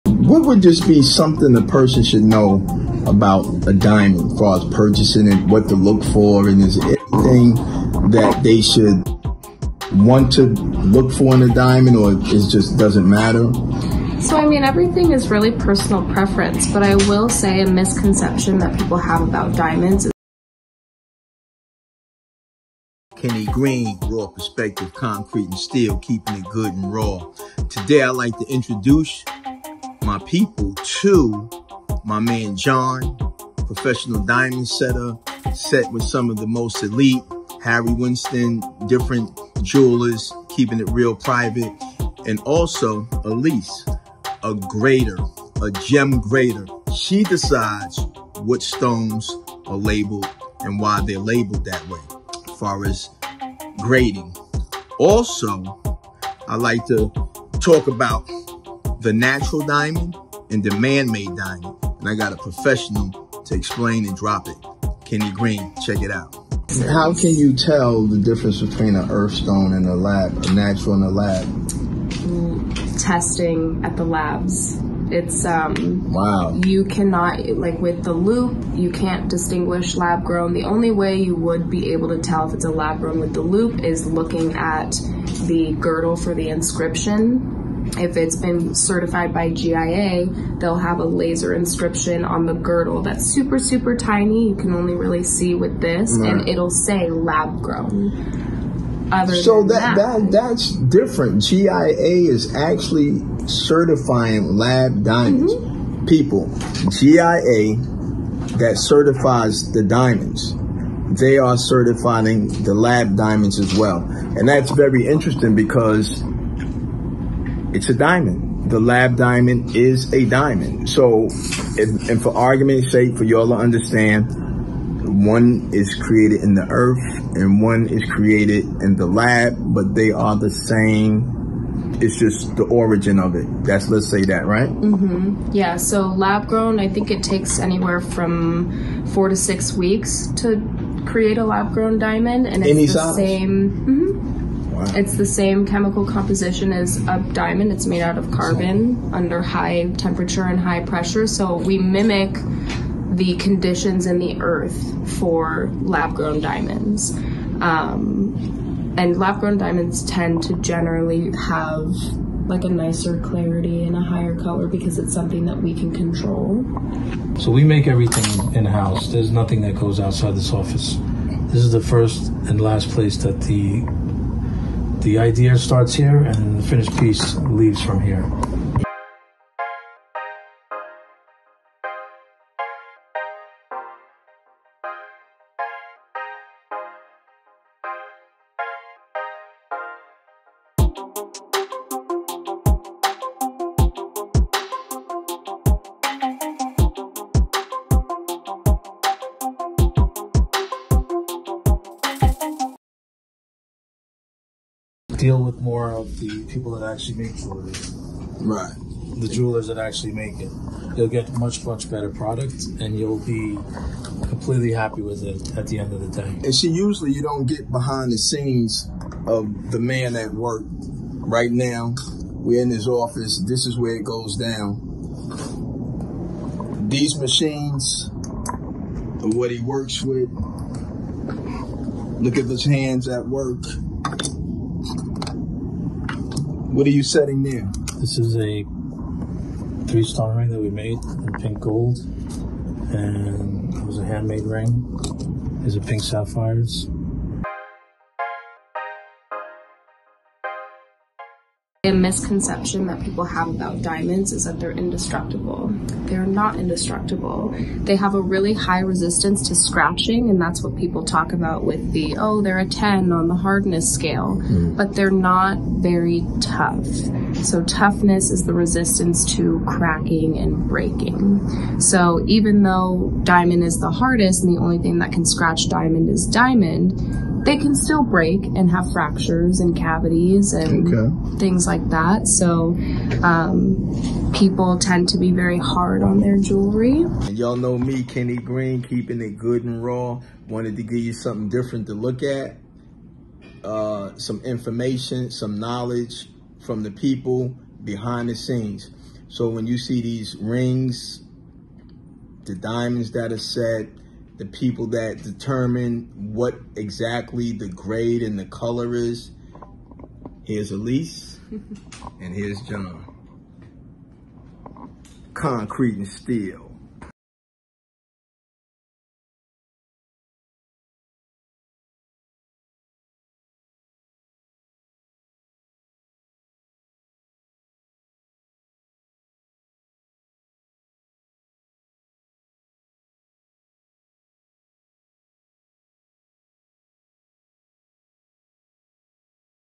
Should You Choose A Lab Diamond? feat. John and Elease from Optimal Innovative Solutions
We discuss lab diamonds vs natural. Diamond District Chronicles